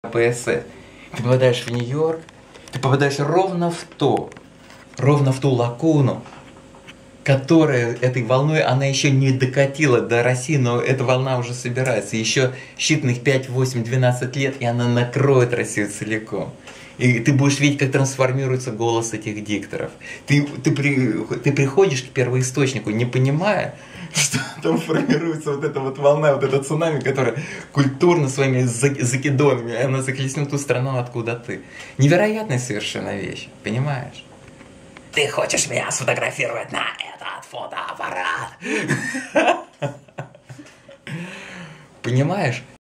Юрий Наумов I
Юрий Наумов о культурном влиянии Америки на Россию